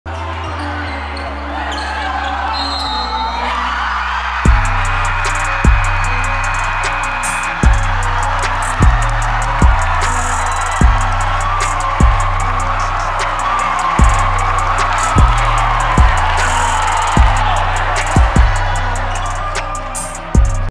PLAY OF THE GAME - Game Winner
Senior Melijah Sullivan hits an unbelievable game winner to seal the deal against Eckerd College and give Tampa the win!